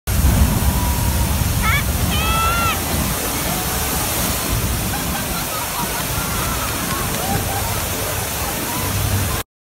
スプラッシュ💦
ジェラシックパーク・ザ・ライドをみて、思わず、カッケー（カツコイー）と、叫んでしました。

前列の人はびしょ濡れ覚悟で、ポンチョ着ていない人もいました。